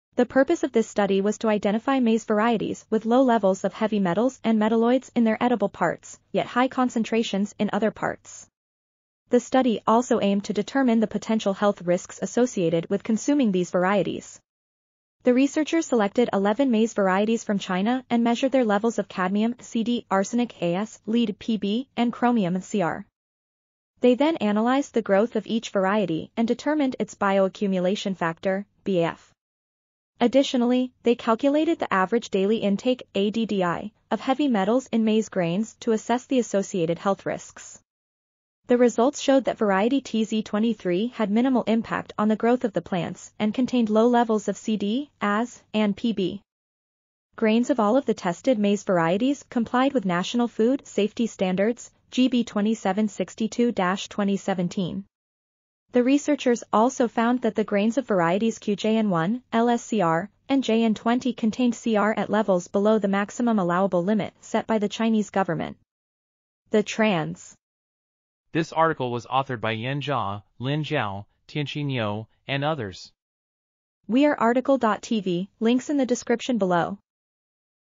Multi-Target Element-Based Screening of Maize Varieties with Low Accumulation of Heav... | RTCL.TV
### Keywords ###

### Article Attribution ###
Title: Multi-Target Element-Based Screening of Maize Varieties with Low Accumulation of Heavy Metals (HMs) and Metalloids: Uptake, Transport, and Health Risks
Authors: Yan Zha, Lin Zhao, Tianxin Niu, Erkui Yue, Xianbo Wang ,and Jiang Shi

### Video Timestamps ###
0:00:00 - Summary
0:01:08 - Title
0:01:13 - Outro
0:01:17 - End